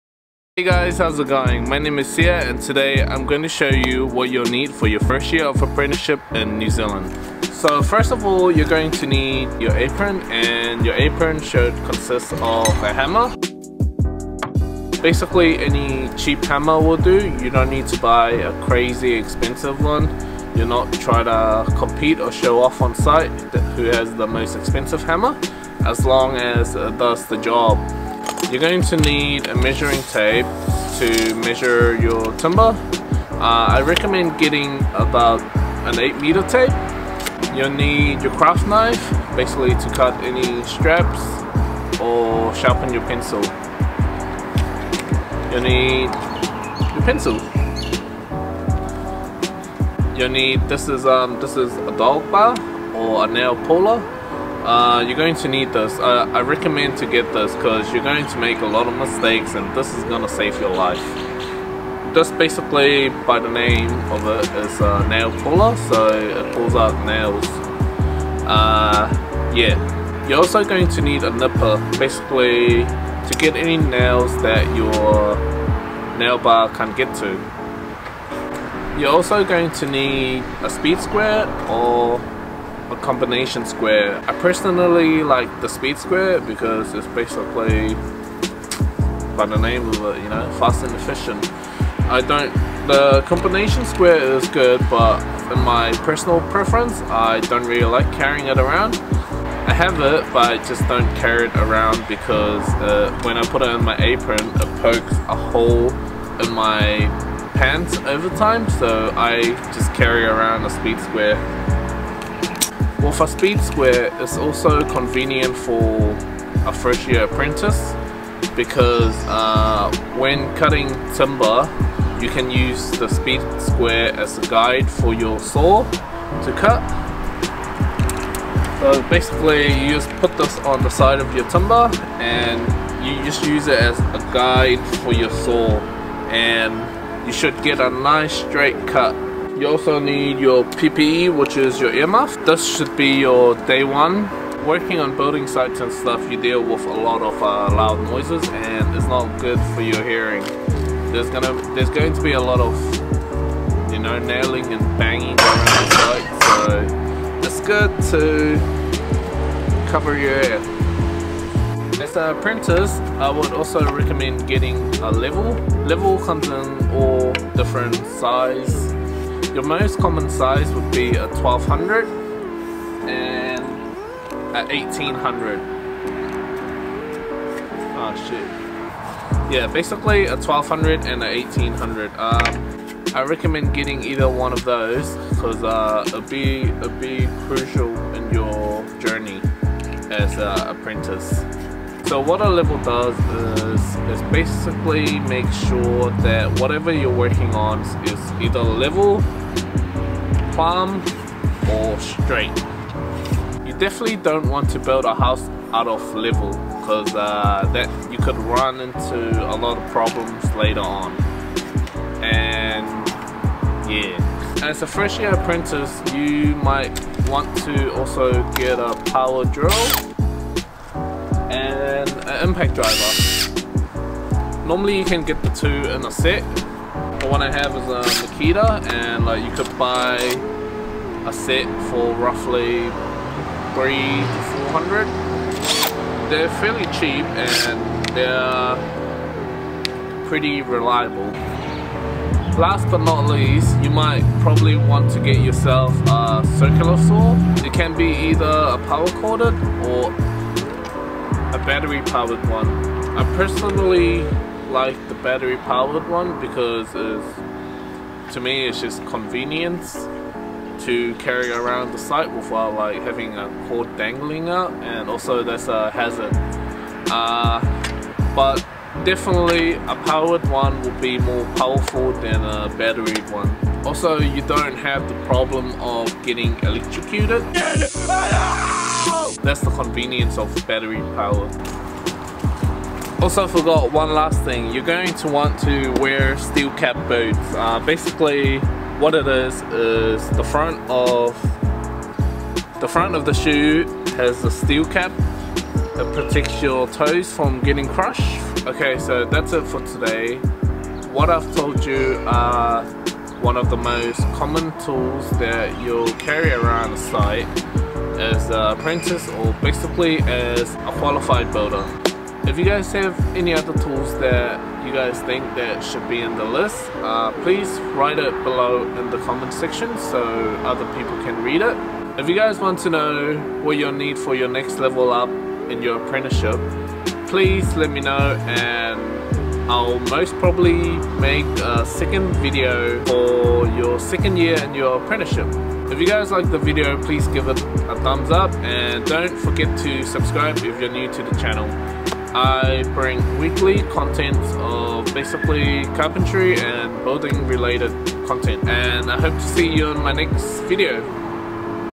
What tools you need as 1st year apprentice
Hey Friends! in thes video il be taking you through one of the most common tools that you should have as a 1st year apprentice, whether you're looking into starting an builder's apprentice or you're already started, or just for entertainment and debate purposes, then you've come to the right channel! in this video I will show you all the basic tools that an apprentice should have or look at getting, these tools will undoubtedly make you more appealing to potential employer and make your job on-site so much easier! it should be your very first investment on your journey to become a qualified builder!

POV: Bored builder at home due to New Zealand lockdown restriction level 4 Delta strain cases.